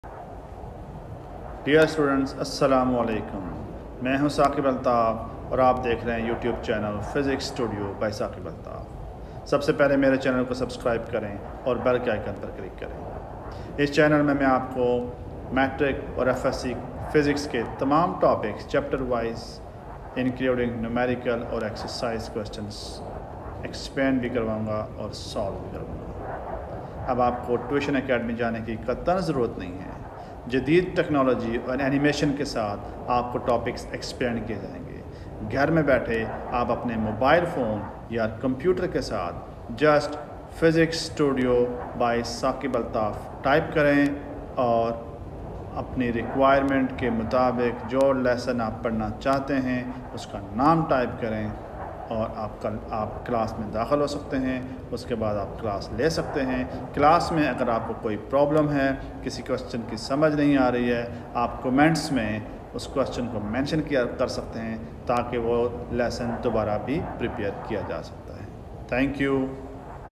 Channel intro
In this channel Matric and FSc Physics related all topics will be explained with animations.